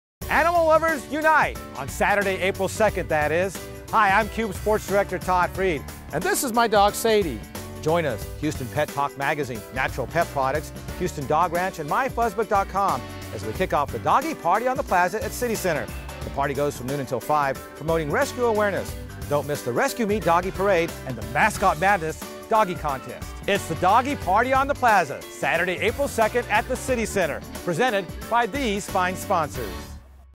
KUBE Doggie Party Plaza revised
Saturday April 2nd the Kube and MyFuzzbook.com present the Doggie Party on the plaza.